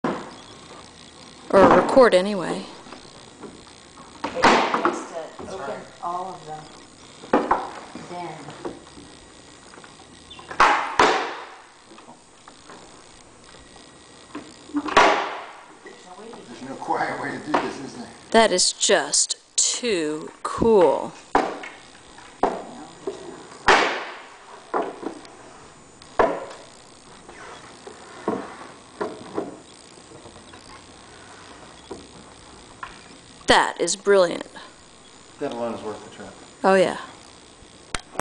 The Amazing Puzzle Table
UPDATE user matthew1796 has given a link to the table's manufacturer . THANK YOU! [/update]
This is one of the coolest pieces of furniture I've ever seen. I went shopping with some friends, and they dragged me over to see this - and I'm glad they did! I have a MUCH simpler dining table that does this sort of thing, but this design is fascinating. If I recall correctly, this is based on an actual table owned by the Spencers of England (yes, of Princess Diana fame...)